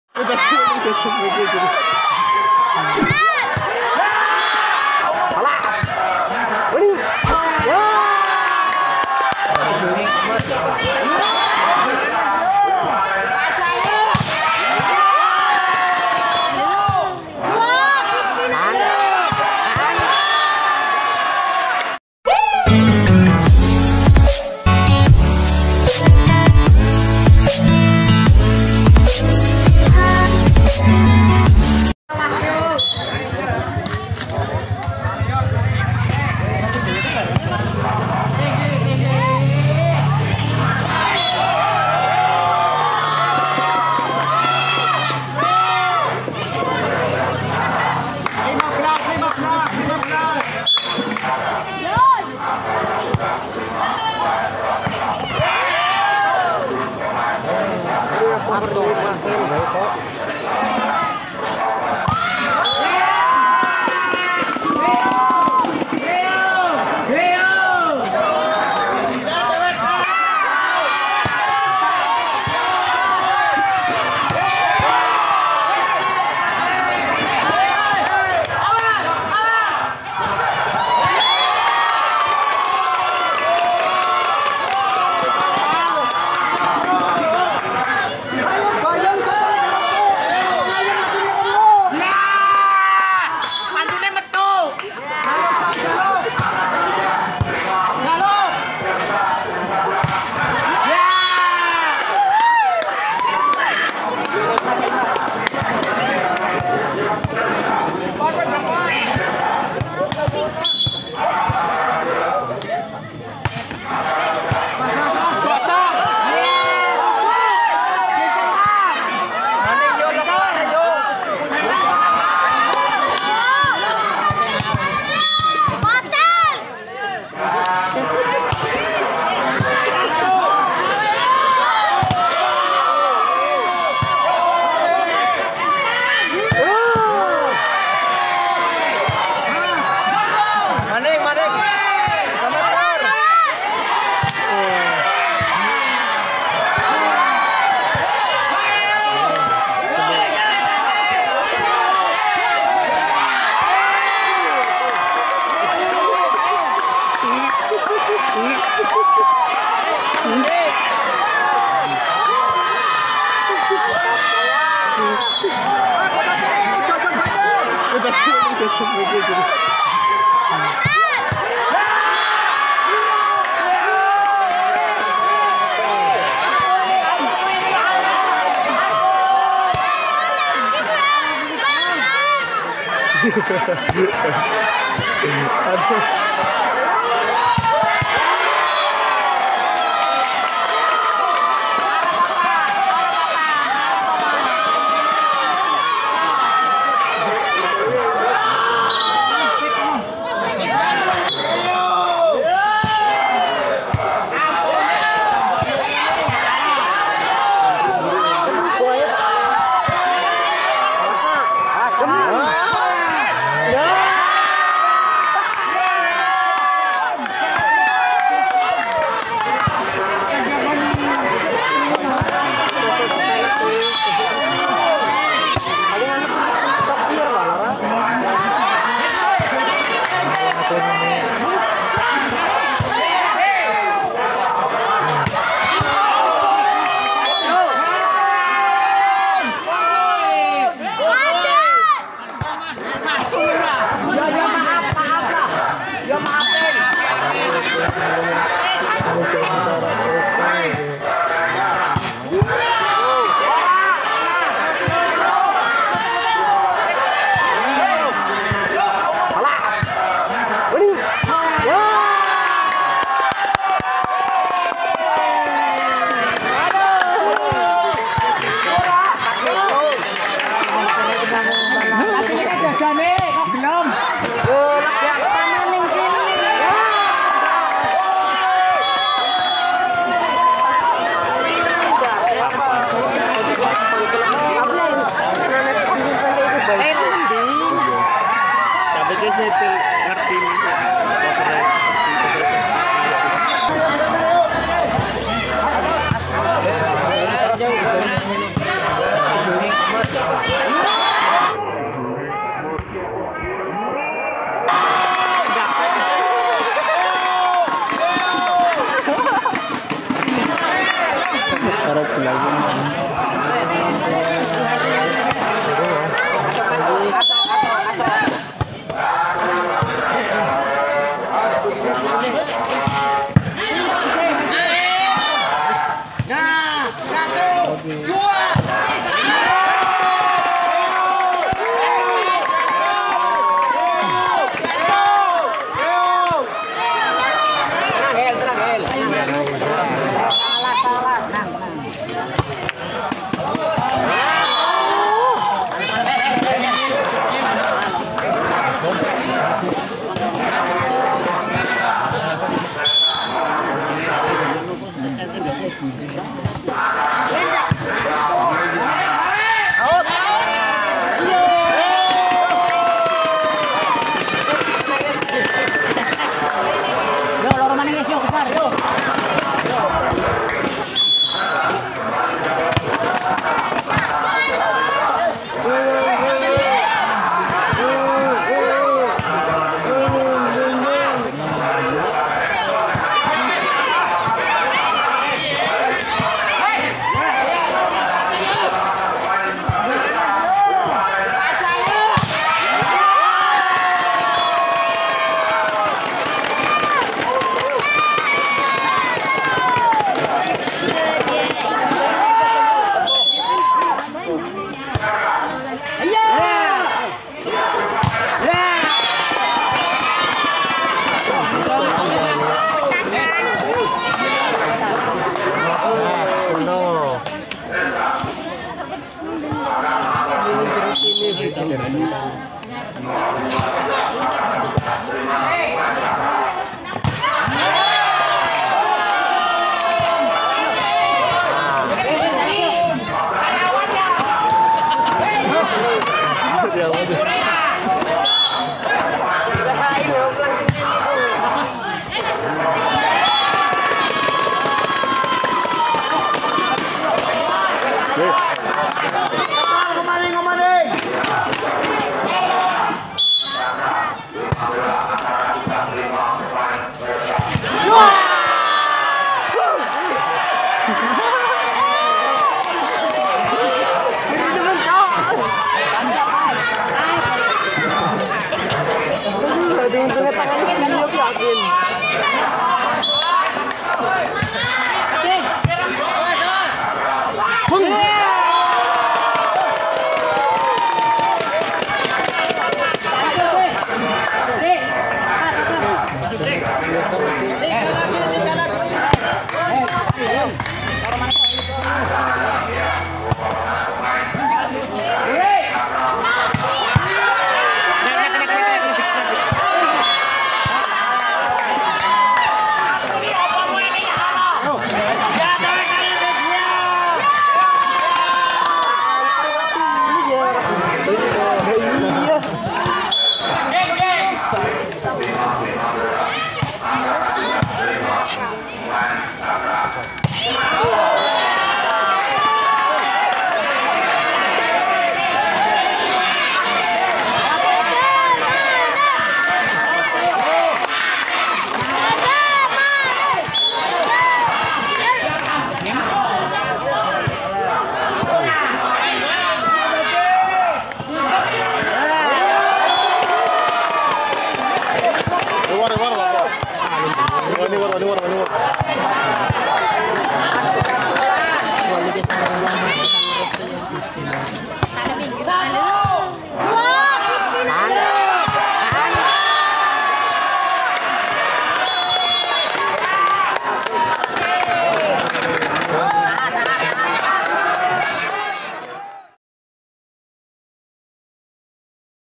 SET PENENTUAN HENDRI DAN DIKA HARUS MENGAKUI KEKALAHAN DARI PEMAIN BANYUMAS🔥 | SEKARJAYA CUP 2022
SET PENENTUAN HENDRI DAN DIKA HARUS MENGAKUI KEKALAHAN DARI PEMAIN BANYUMAS🔥 | SEKARJAYA CUP 2022 SEMI OPEN VOLLYBALL SIRAMPOG